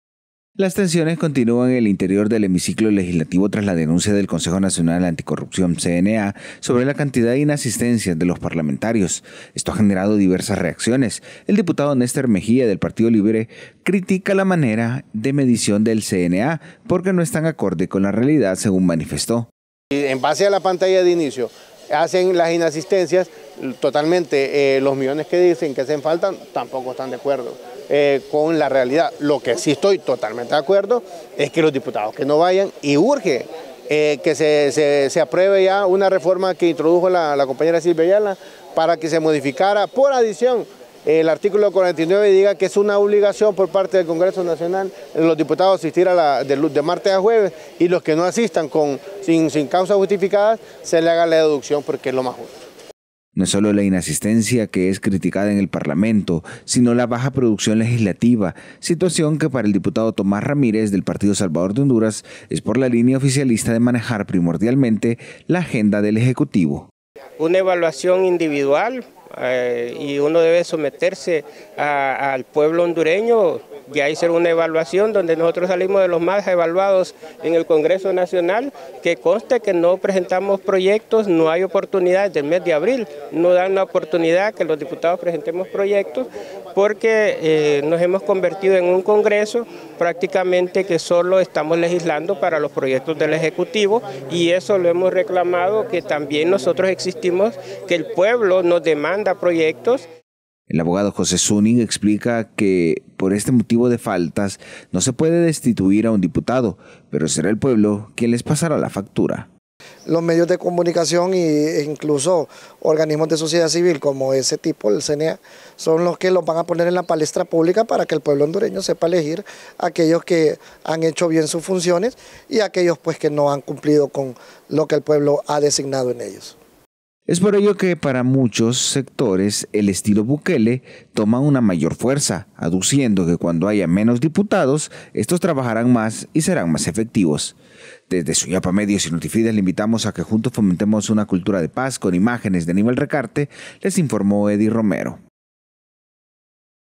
Diputados faltistas
Diputados faltistas no pueden ser destituidos como tal por incurrir en inasistencias, pero si se deberían tomar otras medidas advierten algunos parlamentarios.